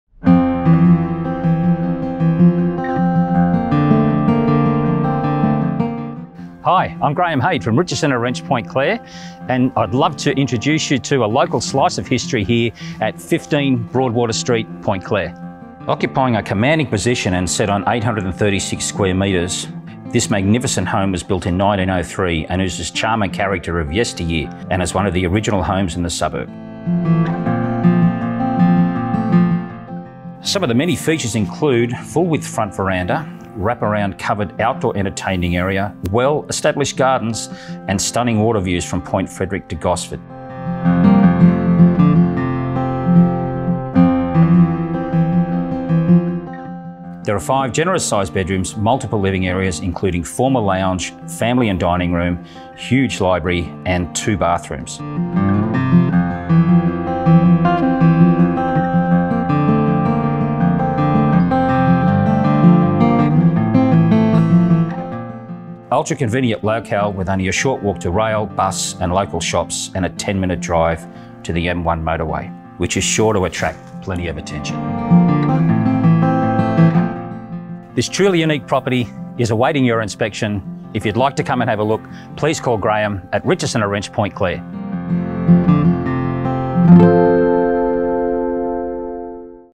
15 Broadwater St Point Clare
15 Broadwater St Point Clare.
An Iconic property with waterviews.
5 bed 2 bathrooms 2 carport spaces
AUCTION - 22nd Jan 2022, 11am, Onsite